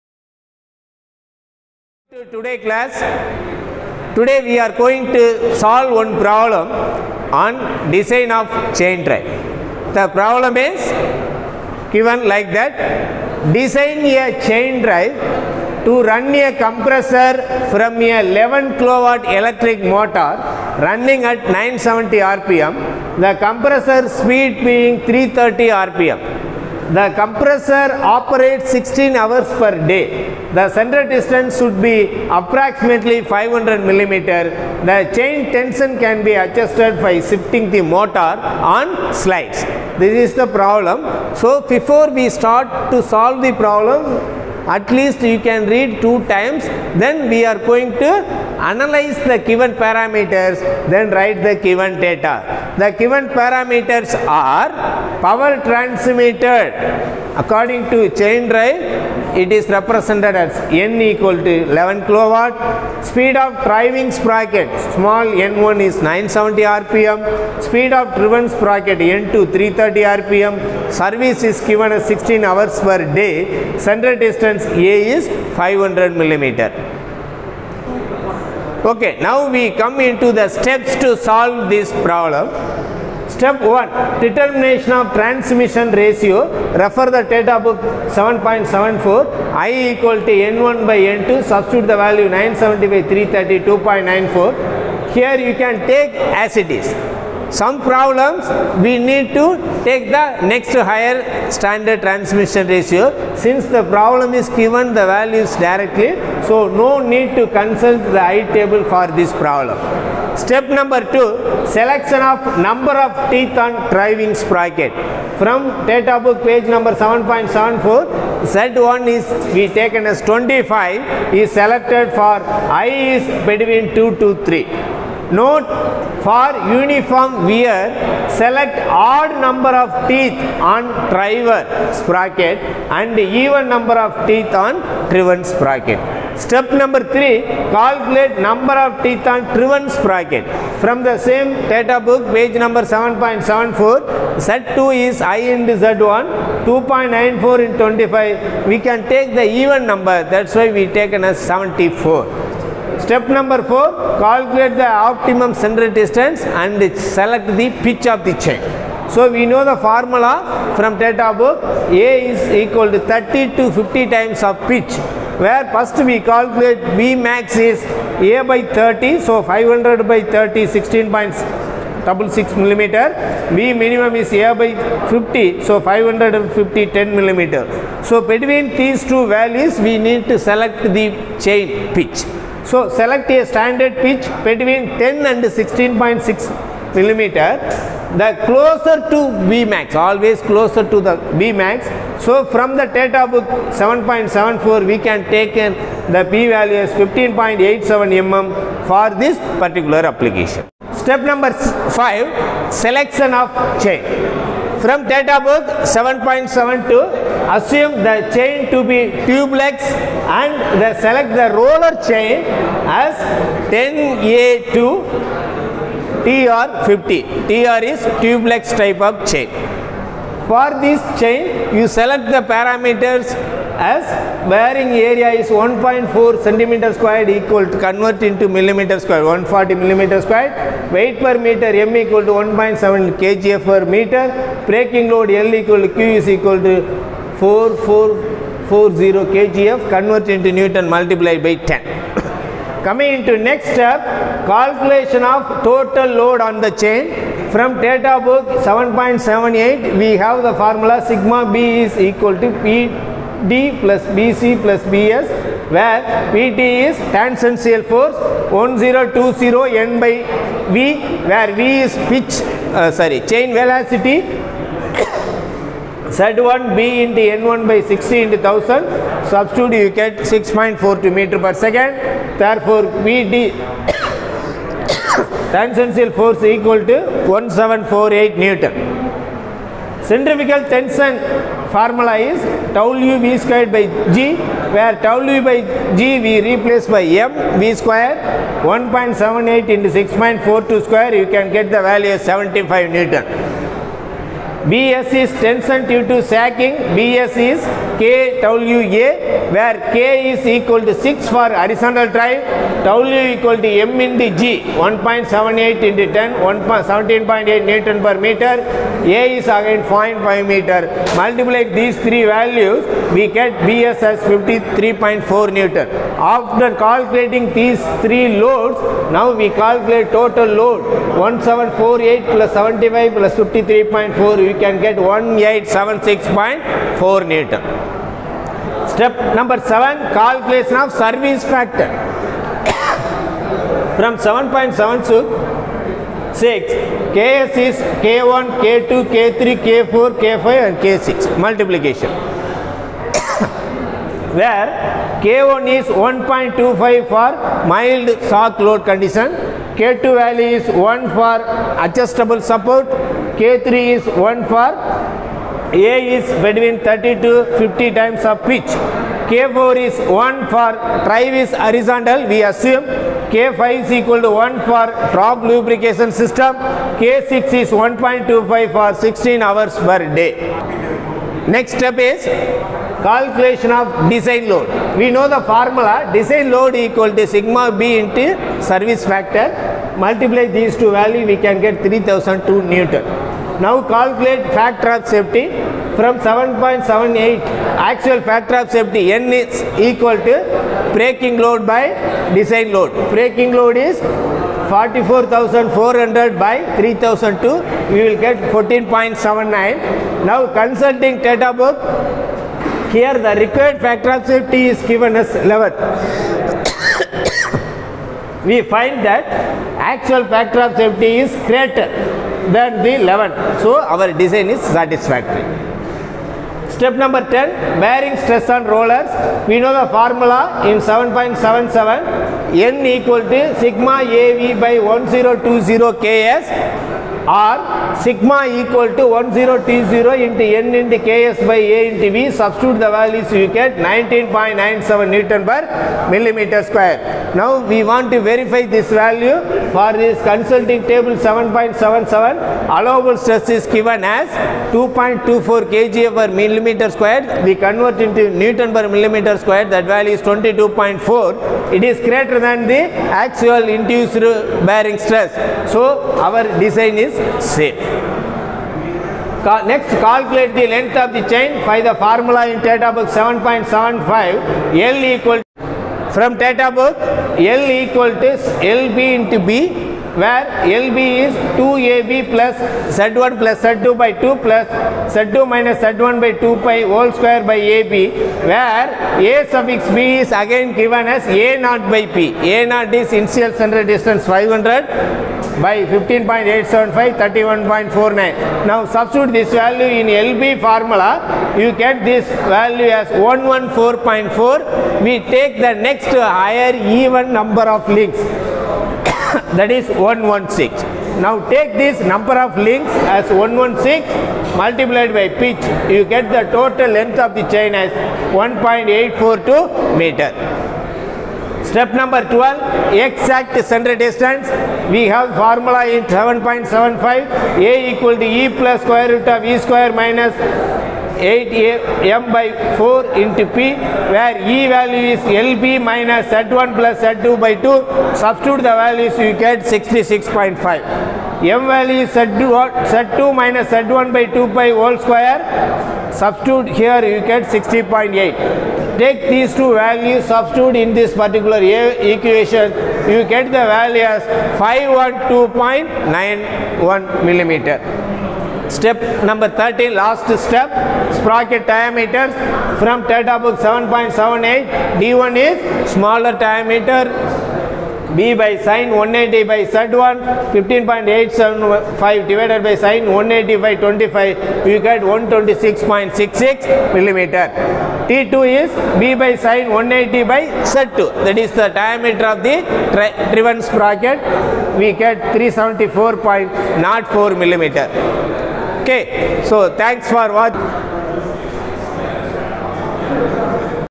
Solving problem in design of Chain drive using manufacturers catalogue PSG Design data
This video  explains the Solving problem in design of Chain drive using manufacturers catalogue  PSG Design data.